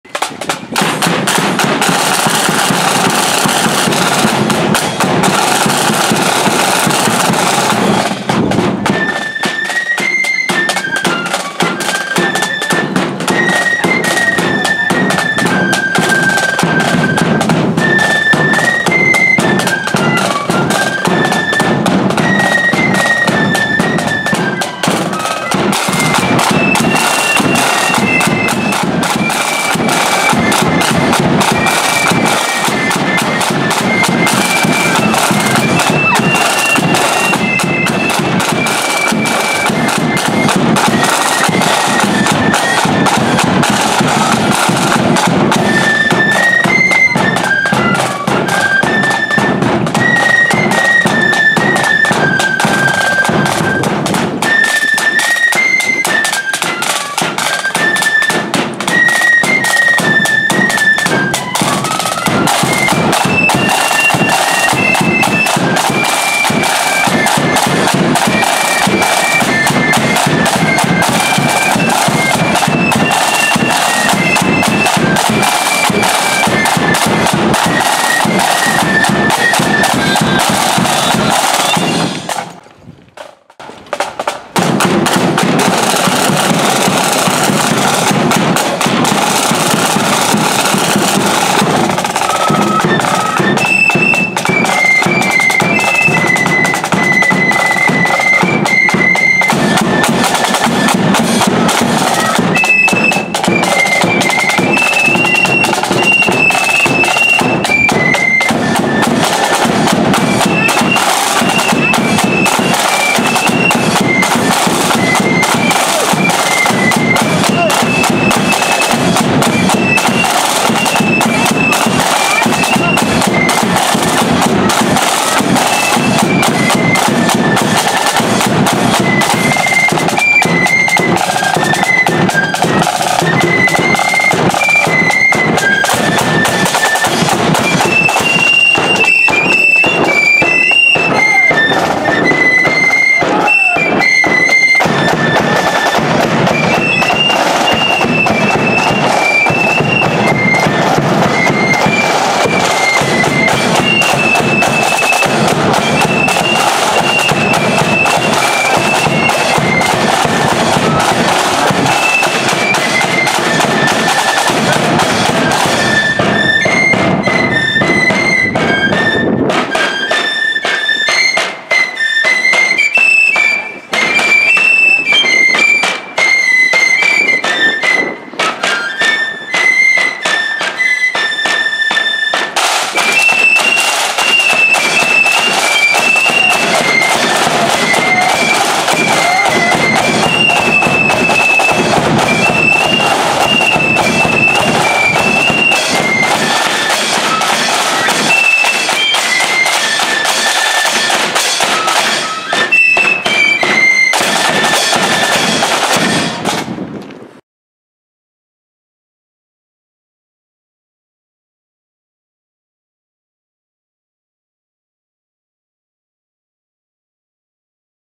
Rathcoole Protestant Boys Flute Band Remembrance Sunday 2019
Rathcoole Protestant Boys Flute Band on parade through Whiteabbey for the annual Whiteabbey Royal British Legion Remembrance Sunday Parade 2019